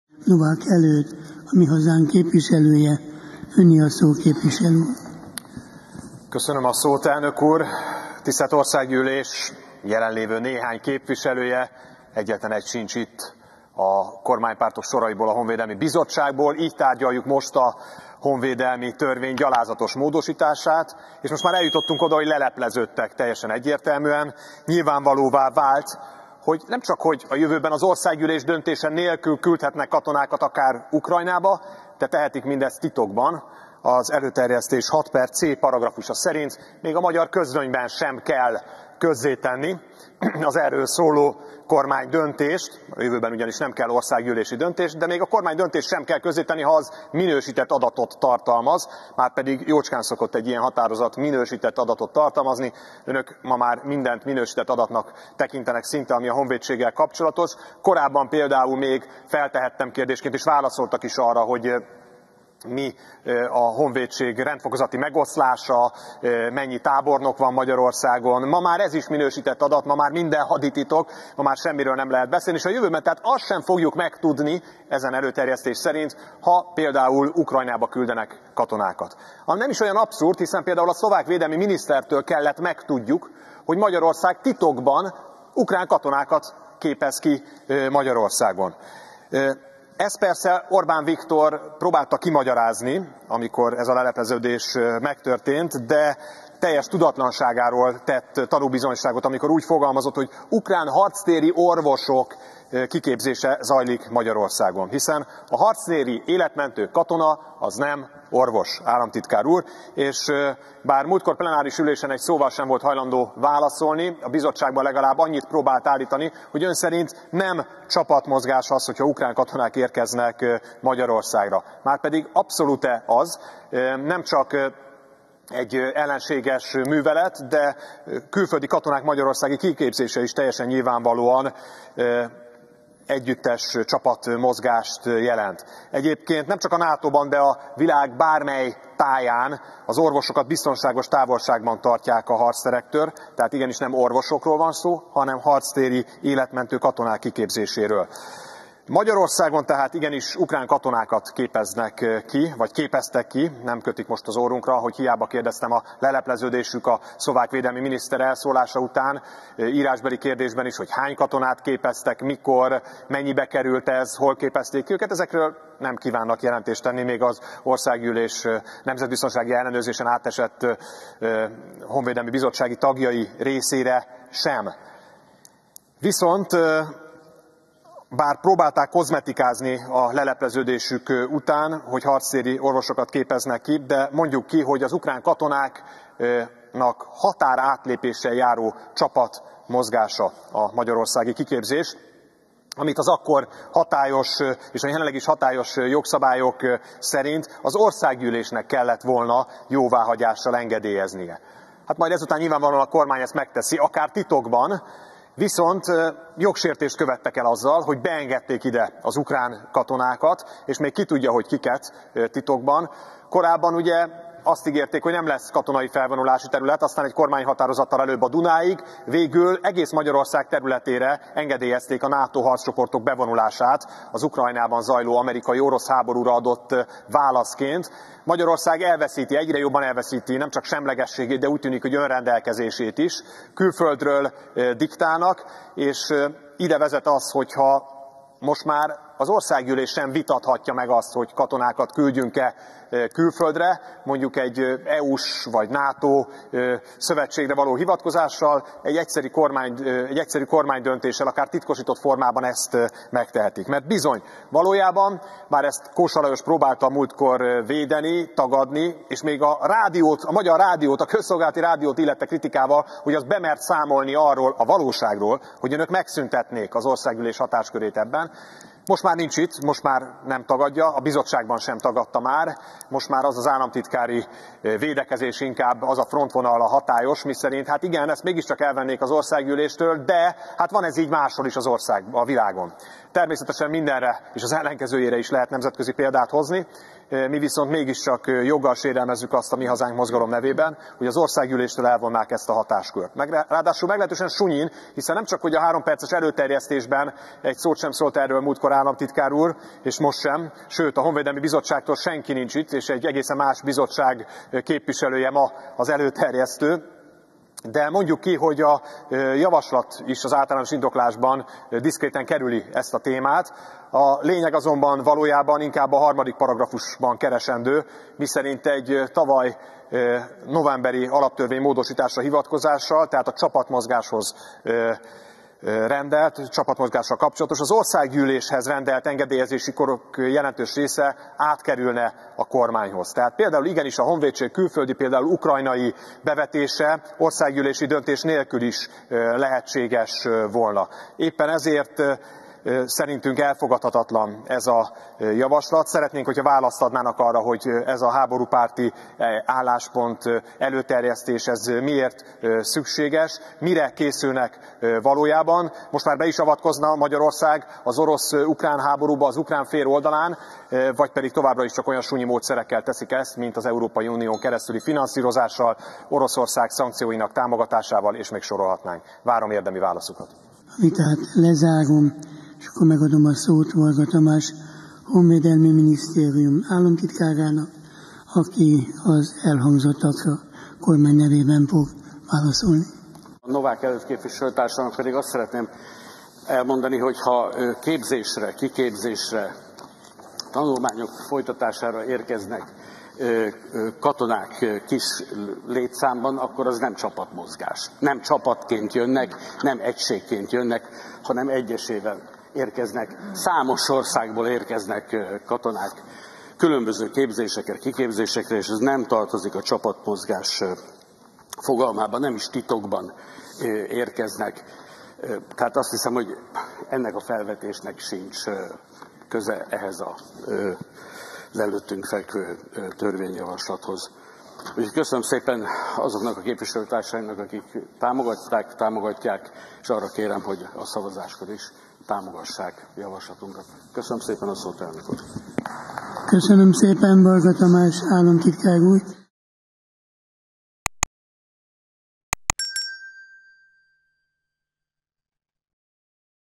Titokban küldhet honvédeket Ukrajnának a kormány ezután
Az Országgyűlés helyett a jövőben a kormány dönthet a Honvédség külföldi, pl. ukrajnai bevetéséről, akár titokban is – világított rá a törvényjavaslat lényegére Novák Előd. Vargha Tamás honvédelmi államtitkár válasza szerint nem is titokban jöttek hazánkba ukrán katonák kiképzésre, melyet csak a szlovák hadügyminisztertől tudhattunk meg utólag, de hogy hány fős ukrán alakulat volt (van?) Magyarországon, azt továbbra sem árulják el.